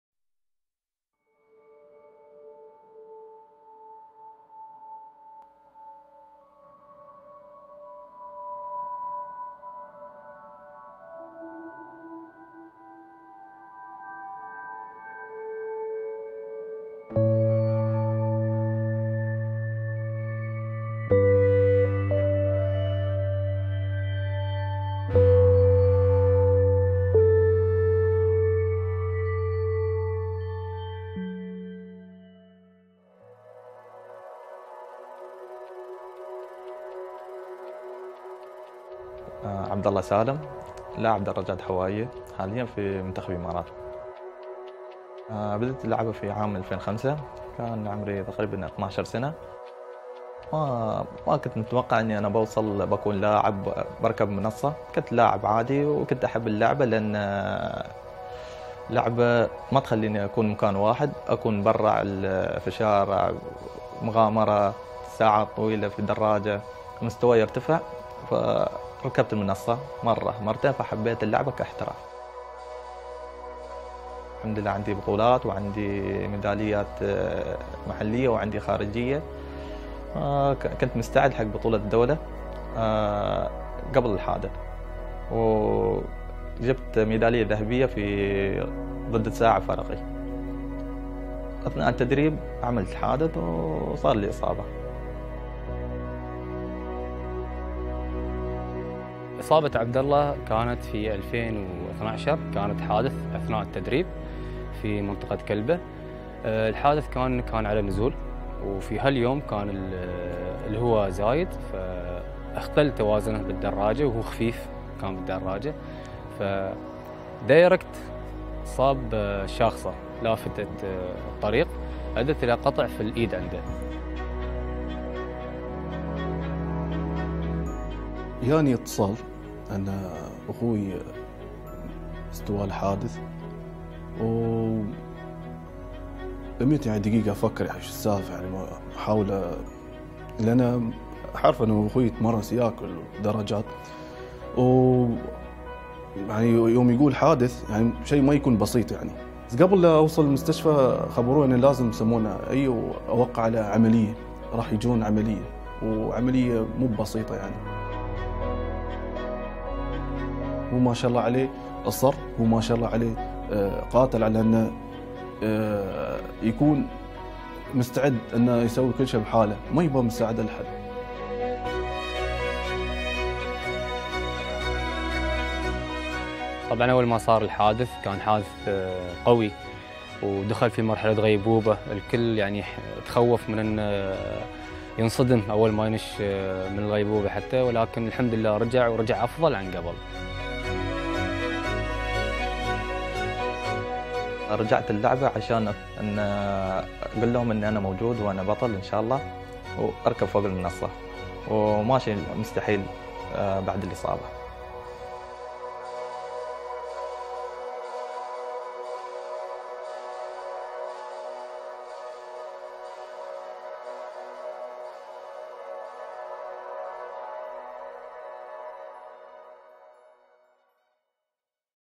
Abdullah Salem al Blooshi is a Paralympic Athlete with an inspiring message
Watch how a tragic accident didn’t stop this UAE National Team cyclist overcame from following his passion.